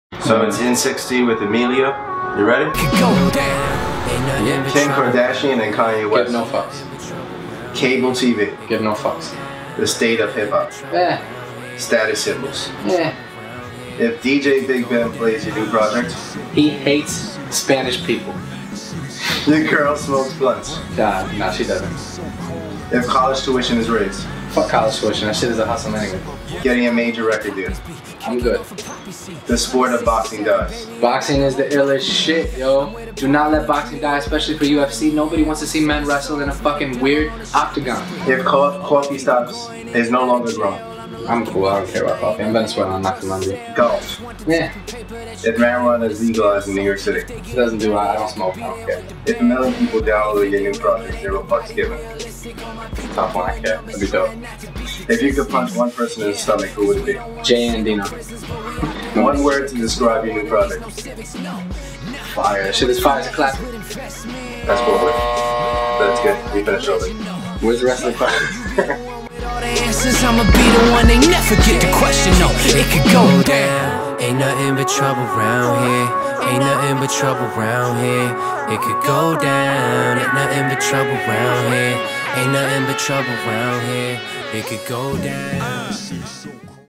MajorStage presents EmilioRojasin60 w/ Emilio Rojas (Interview)
New EmilioRojasIn60 Interview for our new Web series, "In 60" produced by MajorStage featuring Emilio Rojas. Founder of MajorStage sits down with Emilio Rojas to ask a series of random questions and attempts to get them all answered in under 60 seconds.

Video Shot and Edited by Migz @TwinMigz
Interview conducted by Twin @Twinzo
Video produced by MajorStage @MajorStageMusic